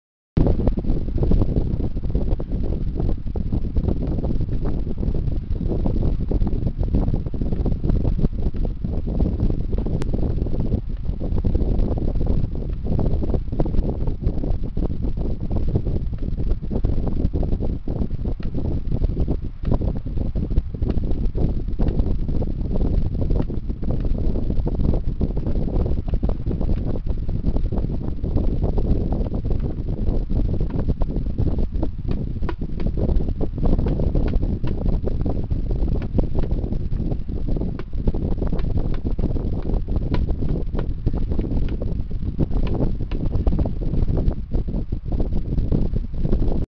Ironman Jaw Hinge
This is our movie accurate Ironman Jaw Hinge system... with the correct PIVOT point.